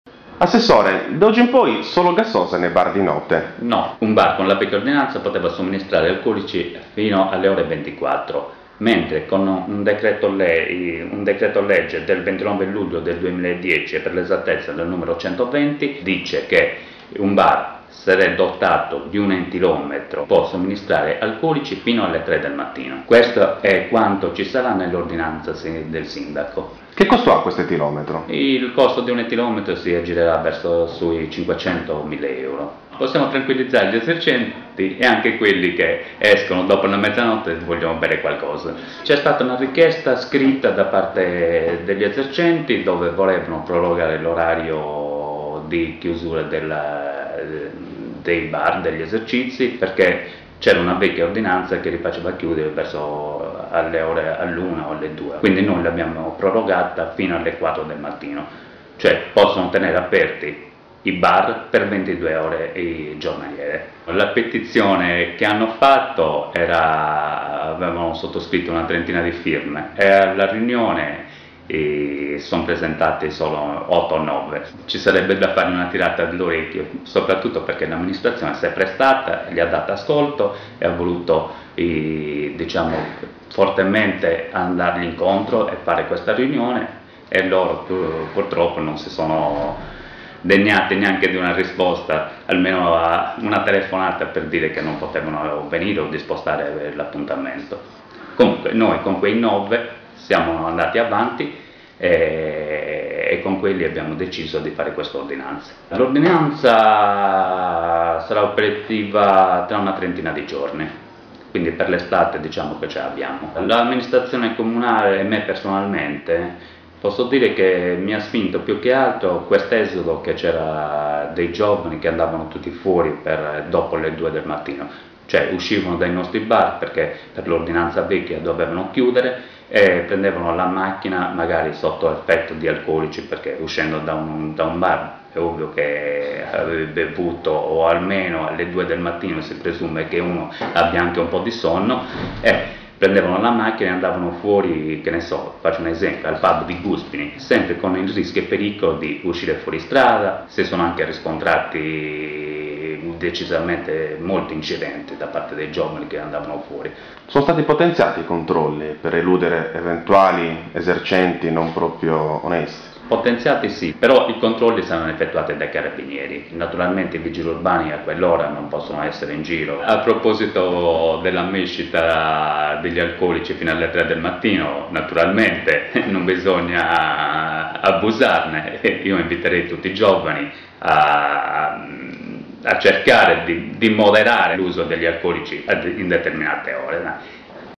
Solo gassosa dopo la mezzanotte? "No, anche alcolici sino alle tre"
Locali aperti fino a 22 ore al giorno e alcolici sino alle 3 del mattino.

Una recente polemica che vedeva contrapposti l'amministrazione comunale e gli esercenti, aveva allarmato il popolo della notte villacidrese, sostenendo che dopo la mezzanotte non sarebbe stato più possibile bere alcolici. Raffaele Loru, assessore al commercio del comune di Villacidro, in questa intervista, chiarisce tutti gli aspetti legati all'apertura dei locali e la mescita degli alcolici.

La nuova ordinanza prevede l'apertura sino a 22 ore per gli esercizi di ristorazione e bar con la possibilità di somministrare alcoolici dopo la mezzanotte a condizione che il locale sia dotato di etilometro da mettere a disposizione di tutti i clienti che volessero verificare il loro grado di alcolemia prima di mettersi alla guida dell'auto, così come previsto dalla legge del 29 luglio 2010 n. 120.

Villacidro.info - 12 febbraio 2011